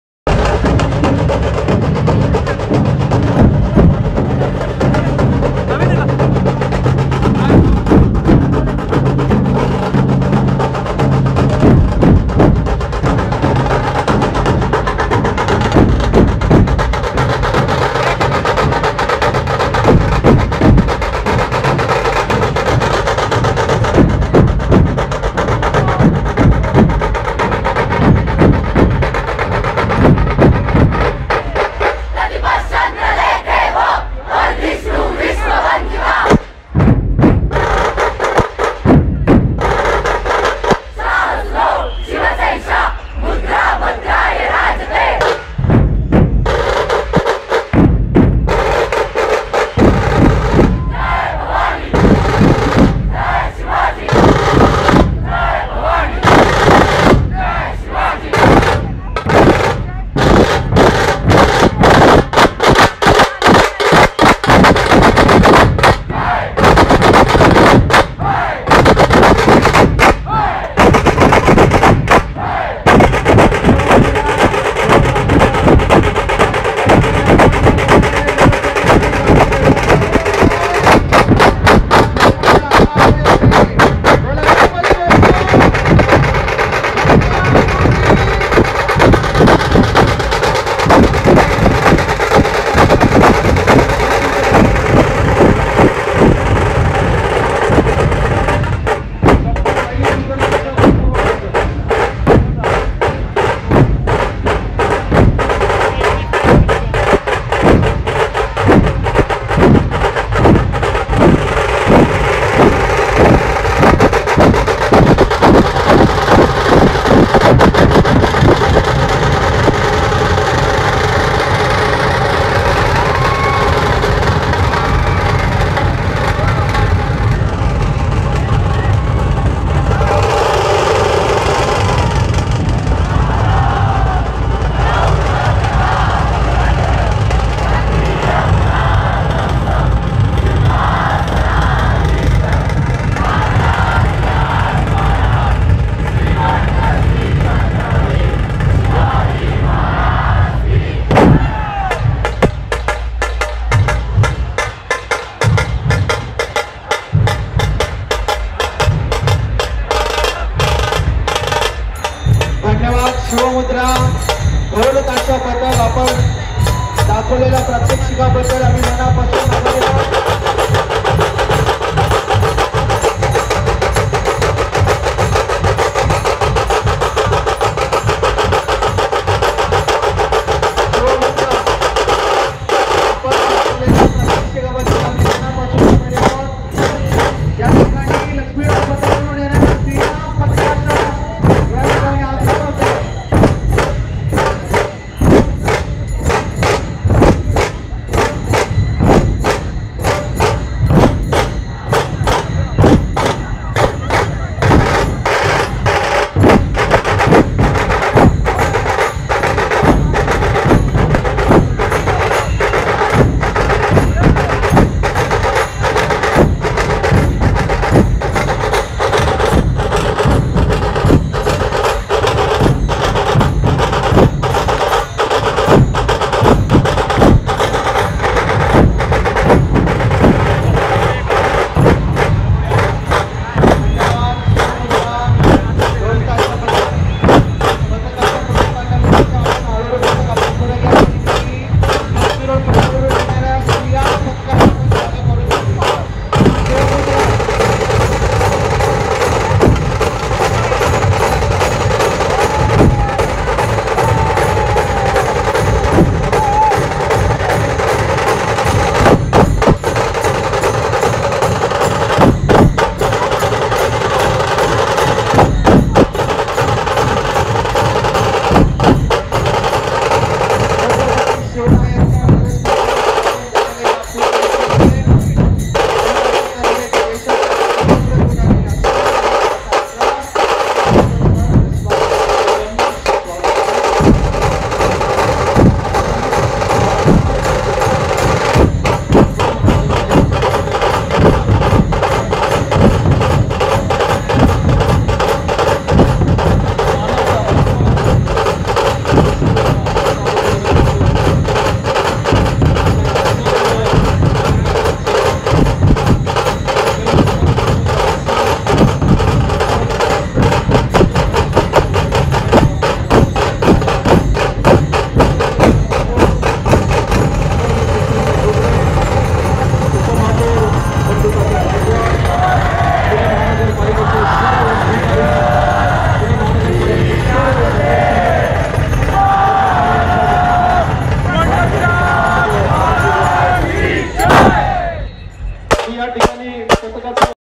Pudchya Varshi Lavkar Yaa...
Video by Anand Chaini

The Dhol-Tasha Pathak group performing in the visarjan procession of Kesriwada Ganpati.

Over 3,500 mandals started their immersion process this morning to bid farewell to the city's beloved Lord Ganesh and mark the conclusion on the 10th day today on Ananth Chaturdashi.